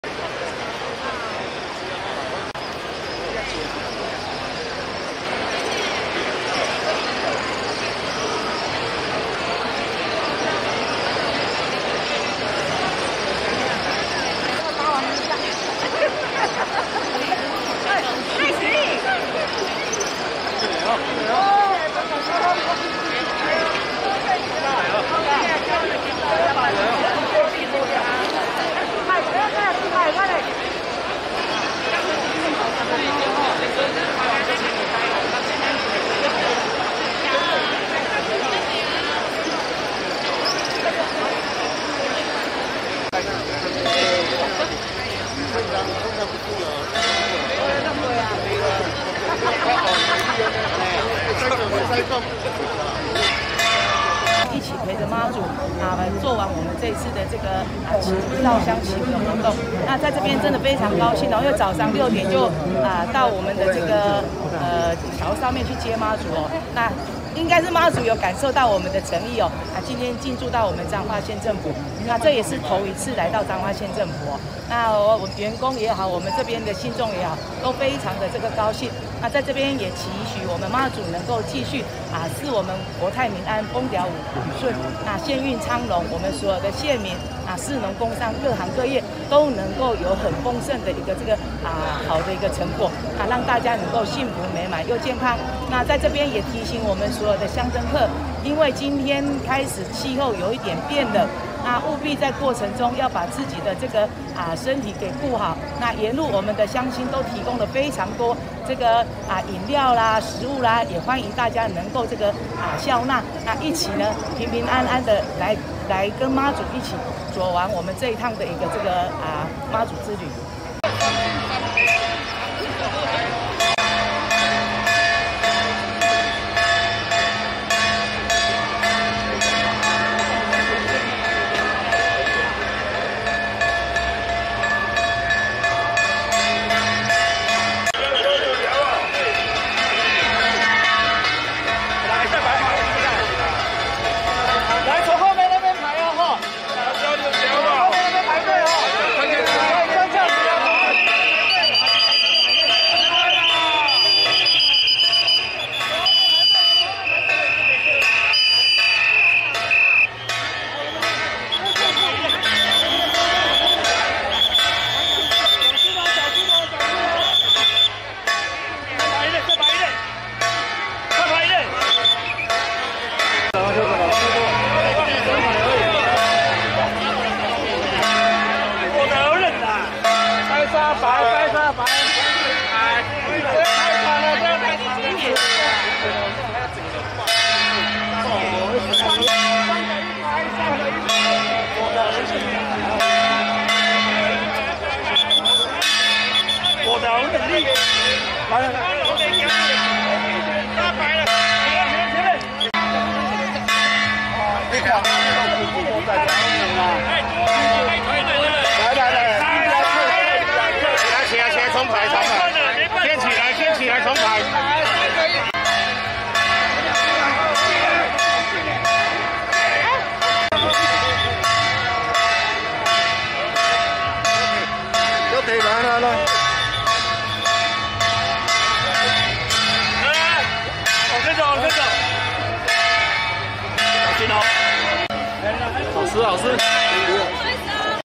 情人節「粉紅超跑」駐駕彰化縣政府
白沙屯拱天宮媽祖徒步進香活動，行動飄忽不定的「粉紅超跑」在彰化縣政府停駕半小時，縣長王惠美跟著信眾一起躦轎腳，還大喊「媽祖我愛你」